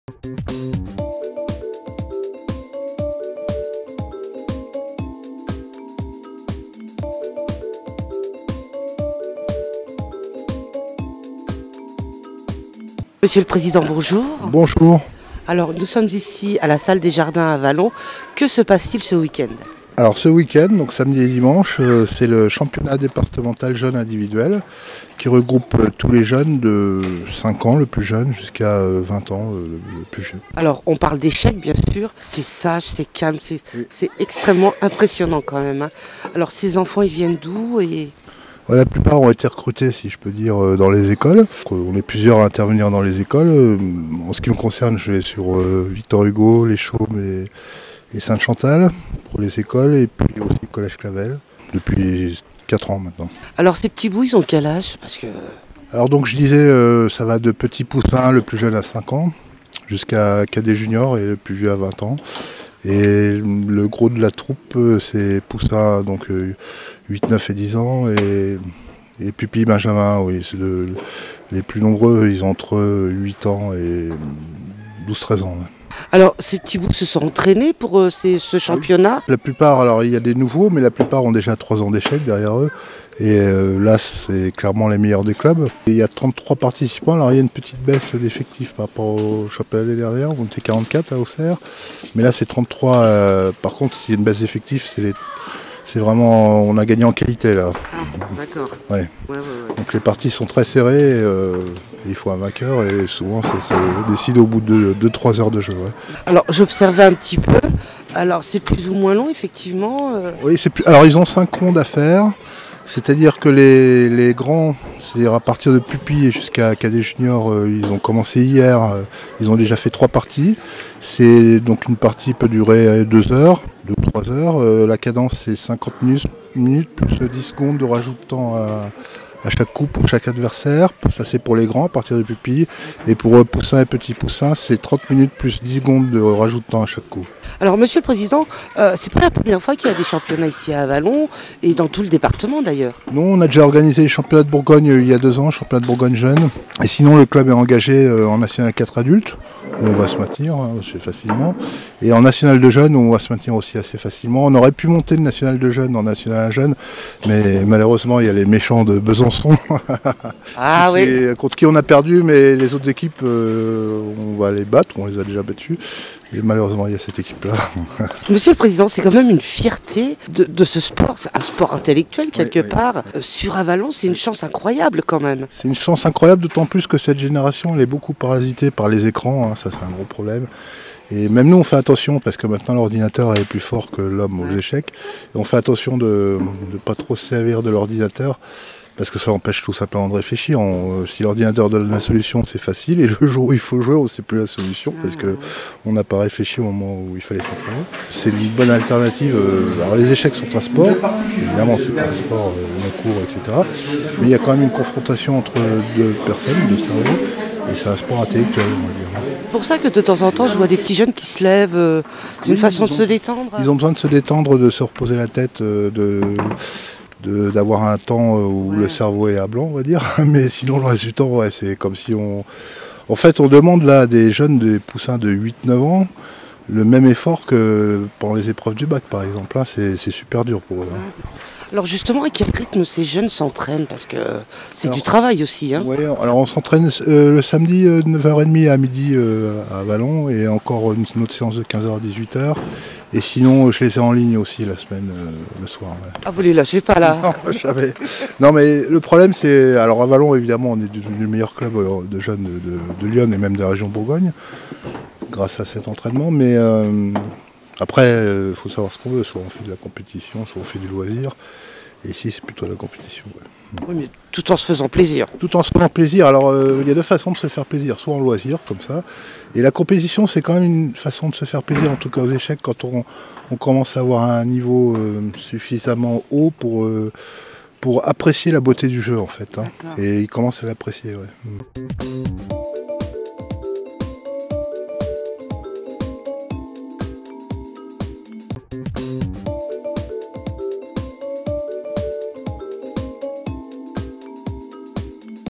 Championnats de l’Yonne d’échecs jeunes à Avallon !
Samedi 2 et Dimanche 3 Décembre 2023, le Comité Départemental d'échecs de l'Yonne organisait les championnats de l'Yonne jeunes, pour les catégories Pupilles, Benjamins, Minimes, Cadets et Juniors.